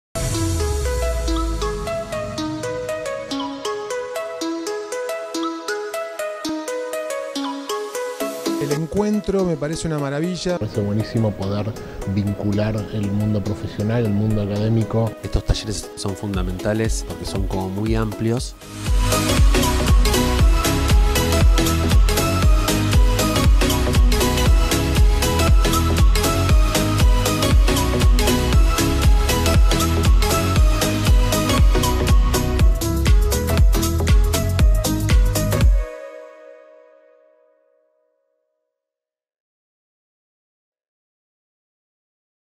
Congreso de Tendencias Escénicas y Audiovisuales 2020, Resumen día 2 - Universidad de Palermo
Resumen del segundo día de la Séptima Edición del Congreso Tendencias Escénicas y Audiovisuales, que se realizó el 27 y 28 de Febrero 2020 en la Ciudad de Buenos Aires.
El evento fue organizado por la Facultad de Diseño y Comunicación de la Universidad de Palermo, de forma libre y gratuita.